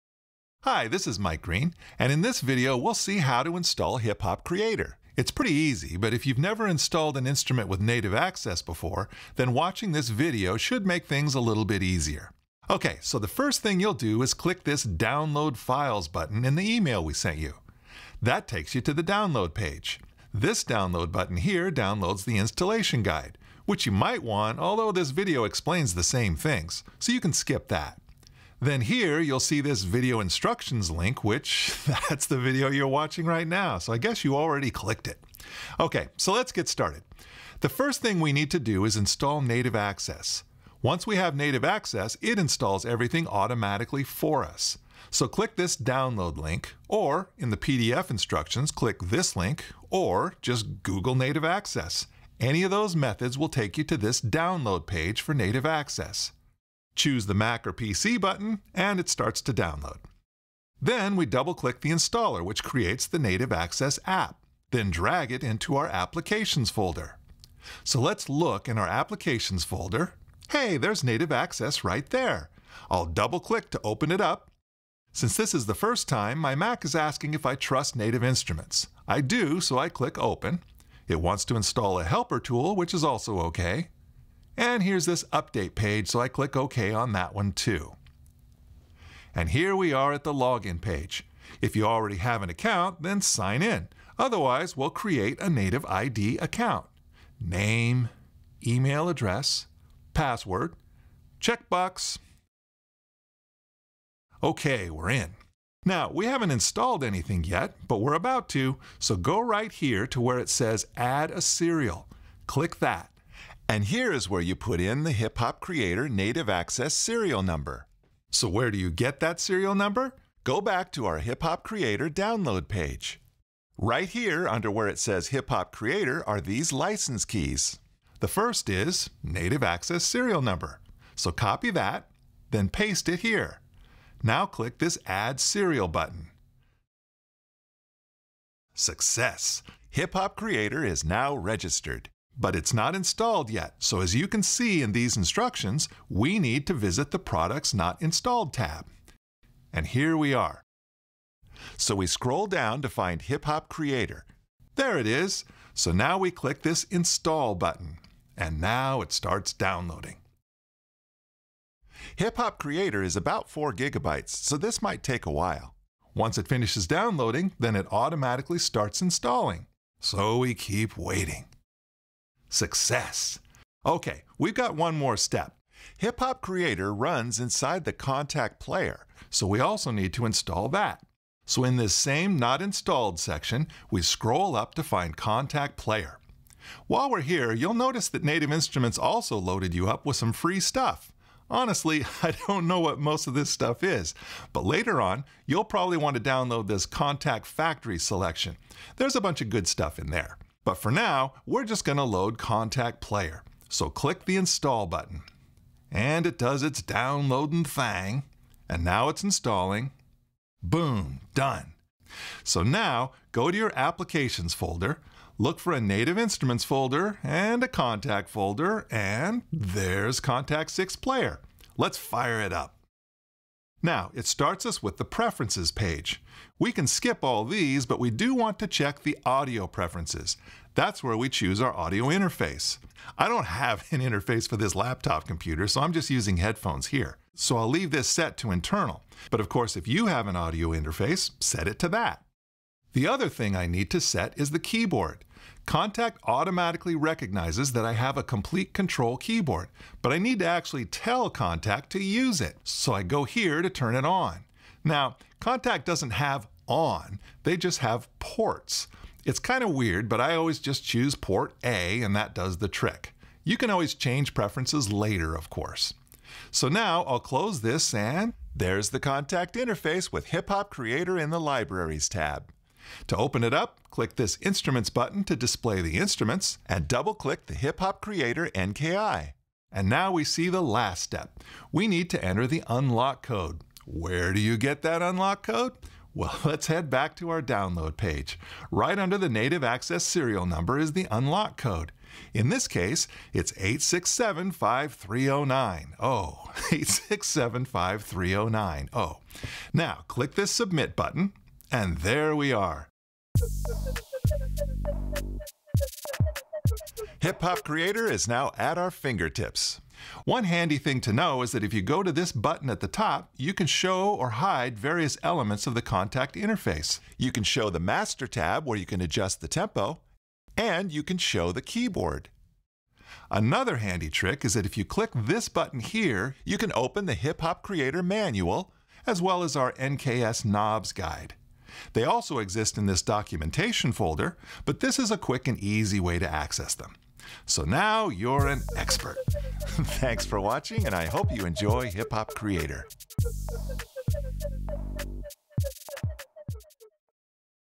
How to Install Hip Hop Creator
This video explains how to install Hip Hop Creator.  Includes instructions on Native Access and Kontakt.